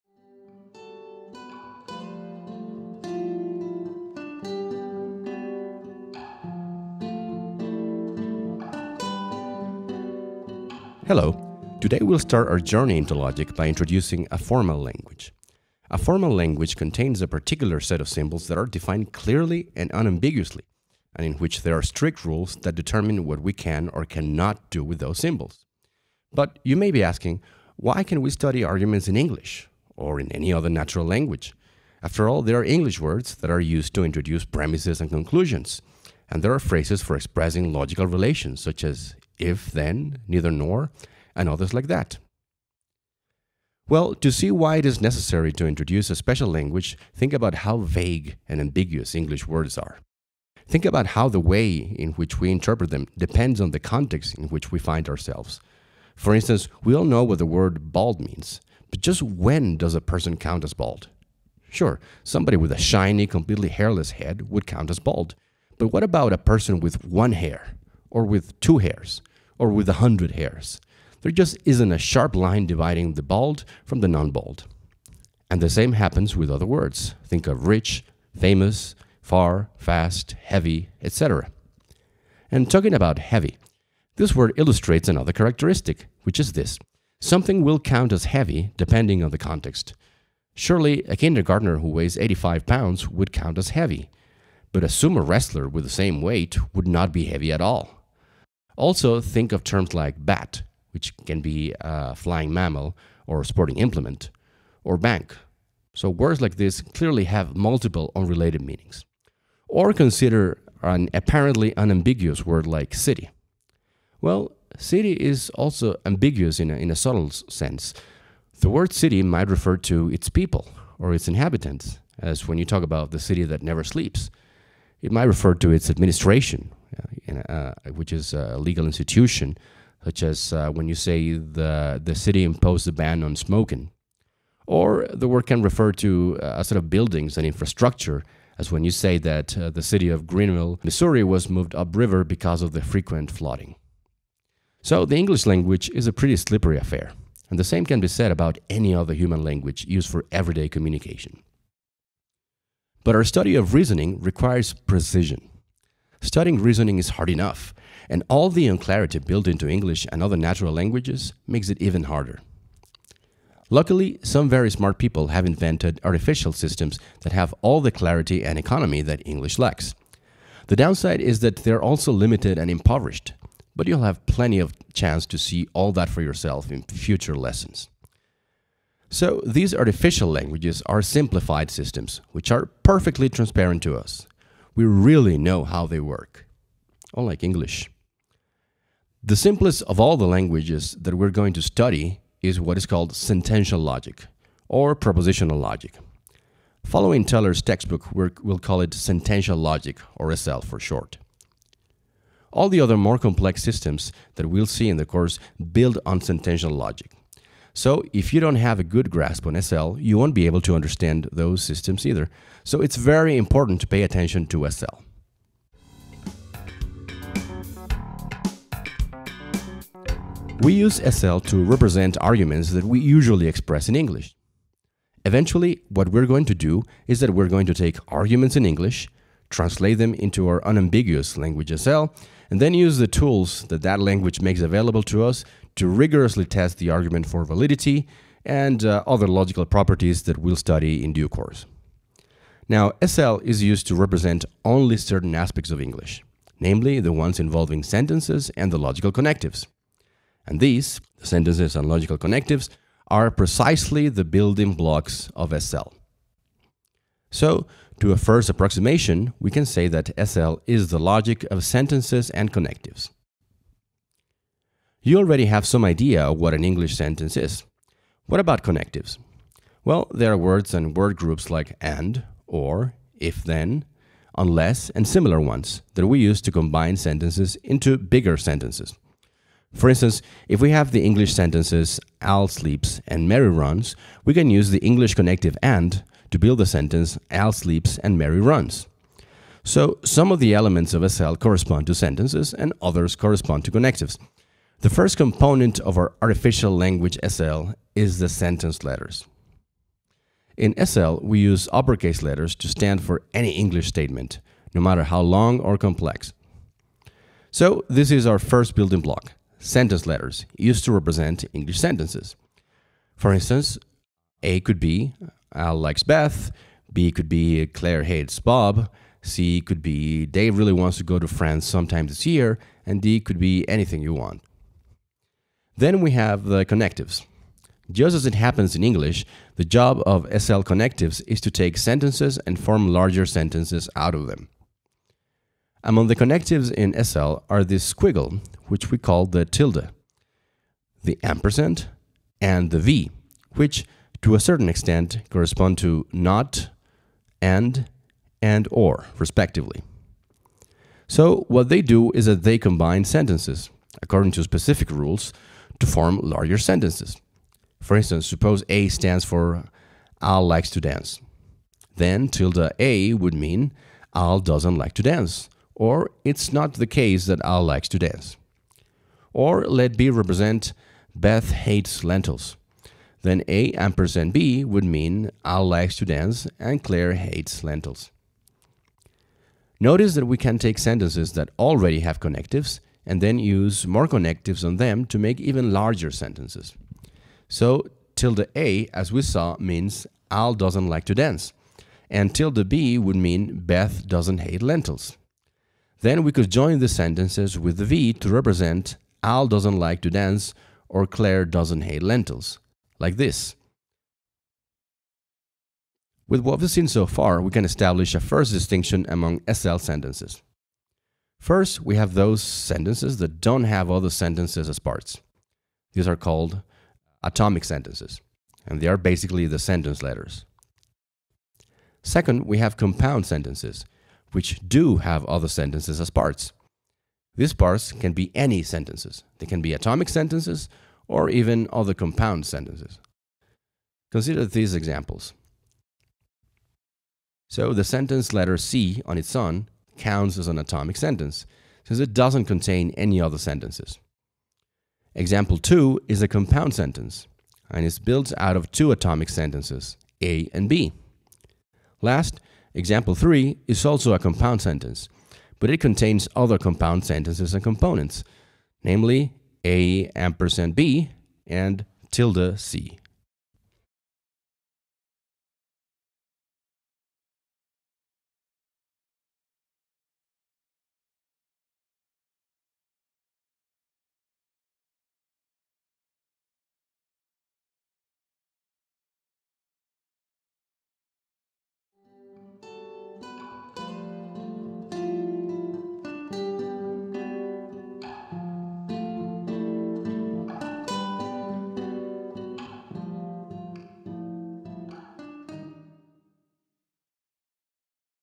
SL1 Sentences and Connectives
A brief introduction to sentential logic (aka propositional logic, with an emphasis on its two main building blocks: sentences and connectives